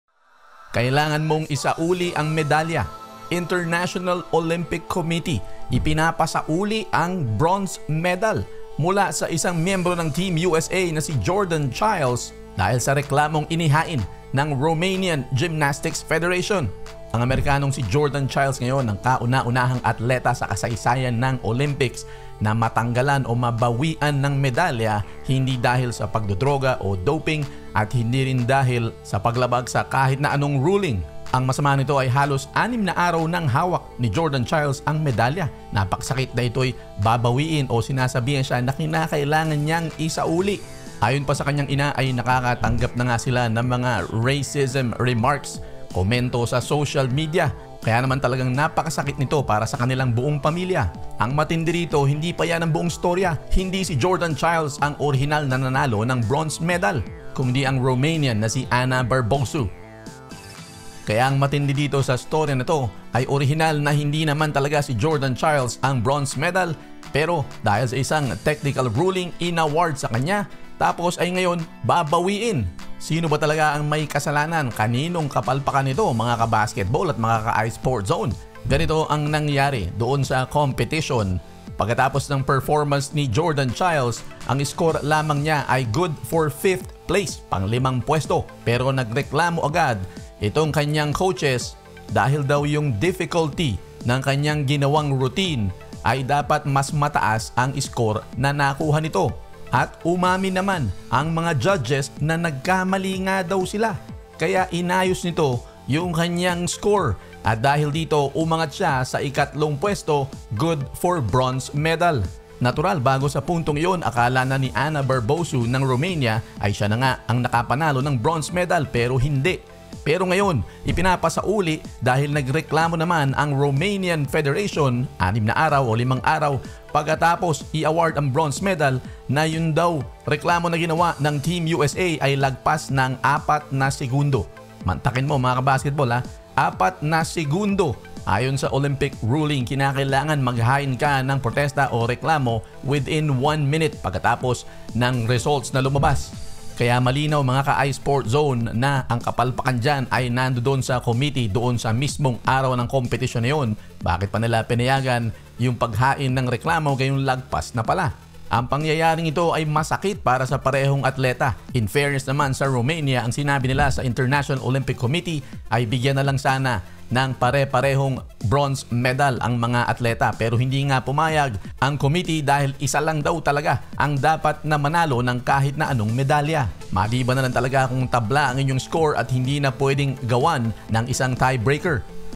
ISAULI MO ANG MEDALYA! Utos sa American Gymnast ng Olympic Committee.
Matinding sakit ang inabot ng USA Gymnast na si Jordan Chiles dahil pinapasauli ng IOC ang medalya anim na araw matapos niyang mapanalunan ito.

1XBet - Get up to 6000 in first time user bonus.